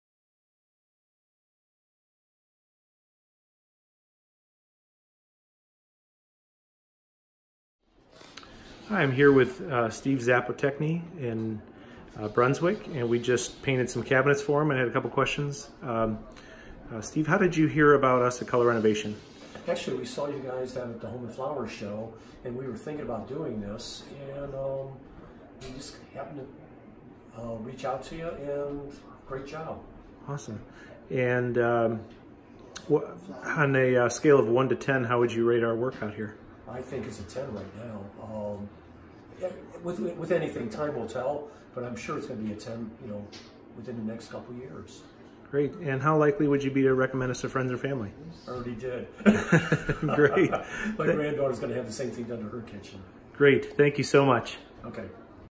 Cabinet refinishing Brunswick Ohio
Steve Z. shares about his experience with Color Renovation. We refinished their cabinets, and gave their dark brown cabinets new life!